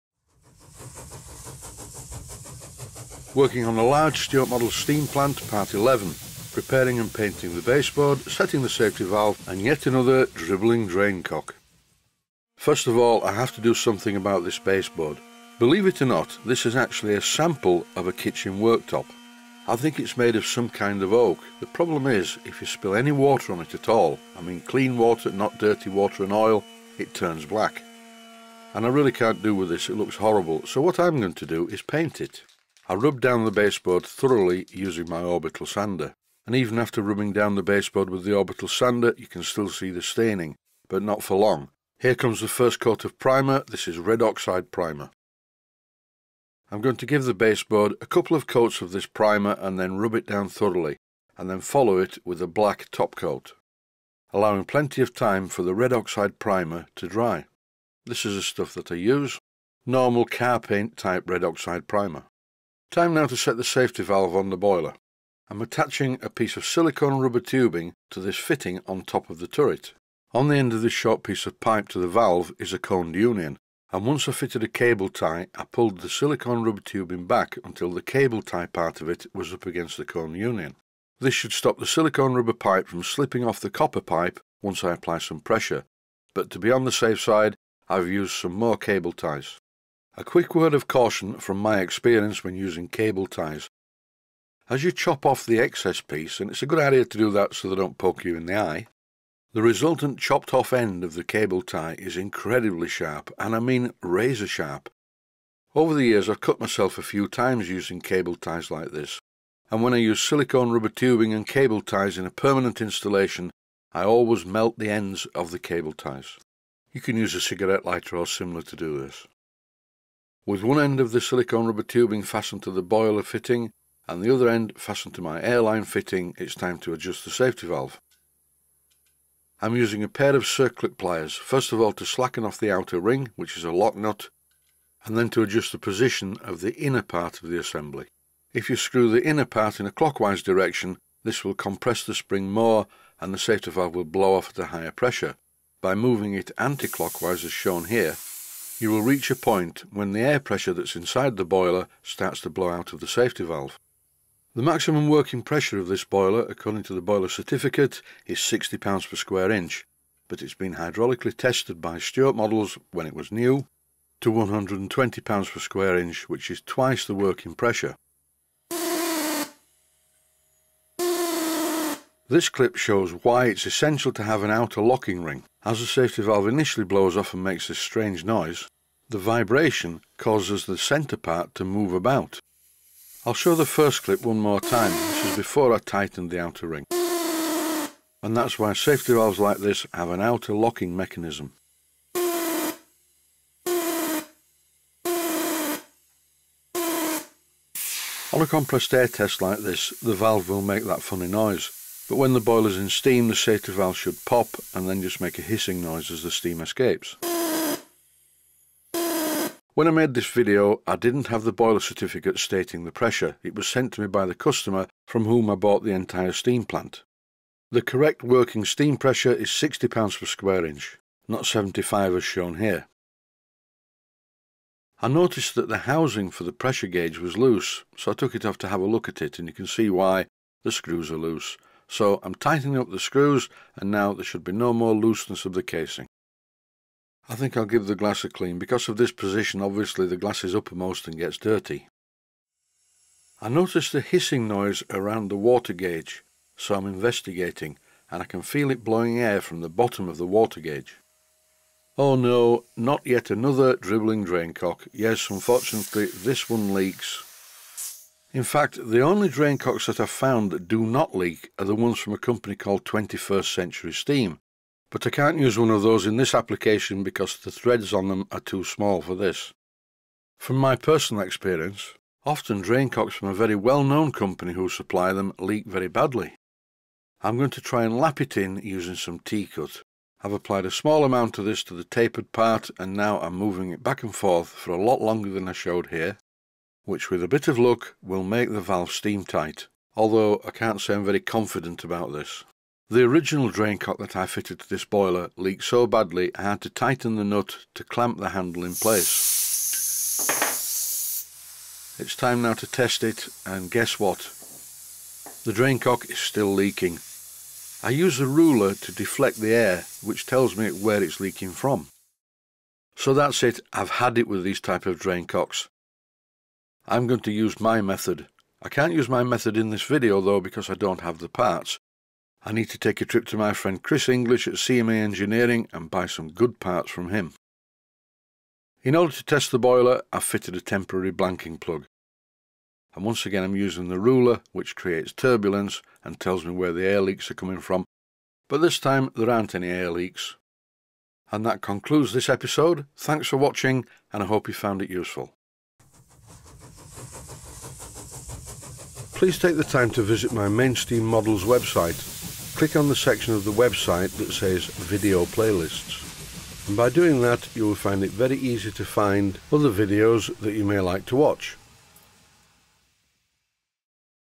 WORKING ON A LARGE STUART MODELS STEAM PLANT - PART #11
Working On A Large Stuart Models Steam Plant - Part #11. Preparing and Painting the Baseboard, Setting the Safety Valve and removing yet another Dribbling Drain Tap .